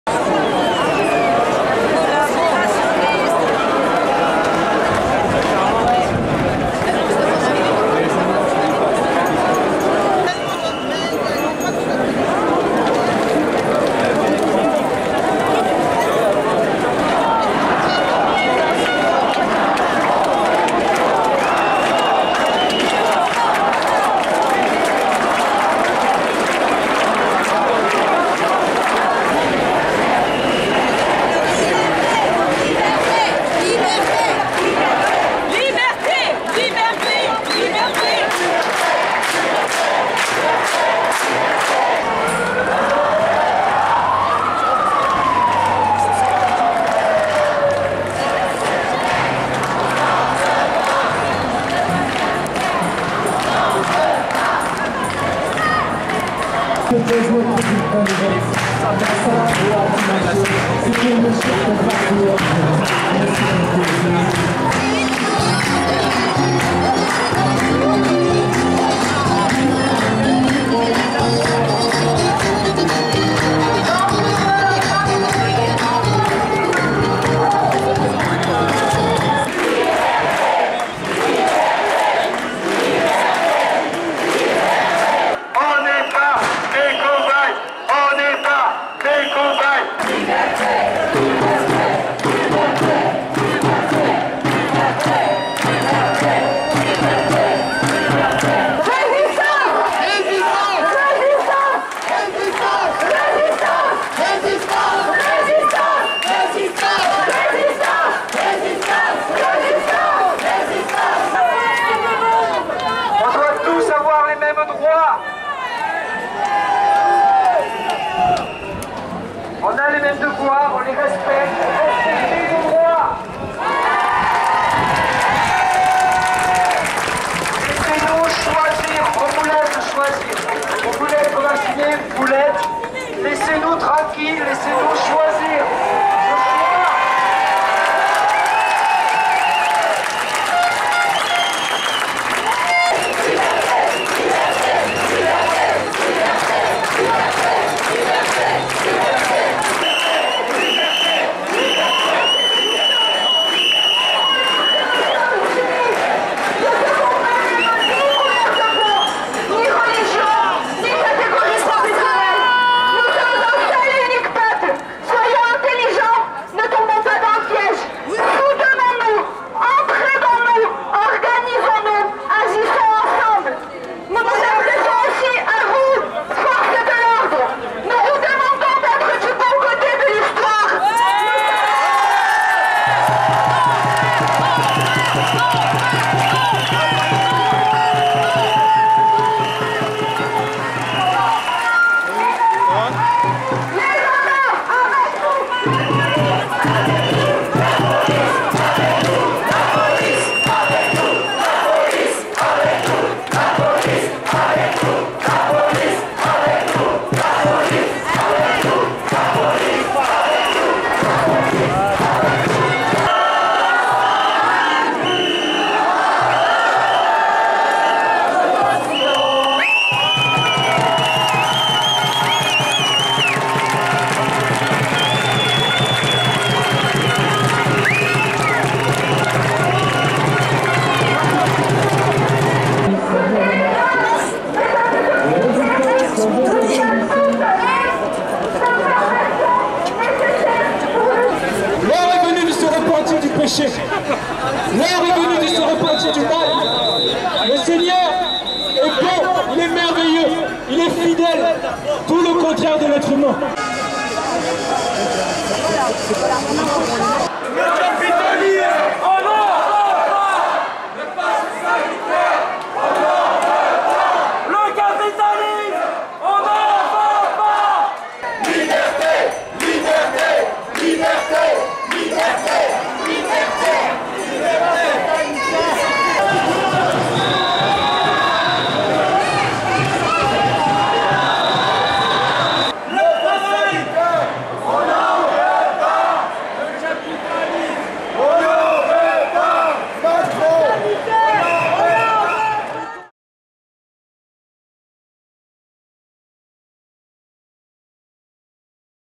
PassSanitaire "Liberté, Liberté!" - Plus de 5000 manifestant·es à Montpellier
Plus de 5000 manifestant·es (source: Préfecture de l'Hérault) ont déambulé ce samedi 17 juillet dans les rues de Montpellier, comme des dizaines de milliers en France, pour protester contre l'extension du Pass Sanitaire aux activités quotidiennes, et dénoncer la politique sanitaire menée par le président de la République Emmanuel Macron.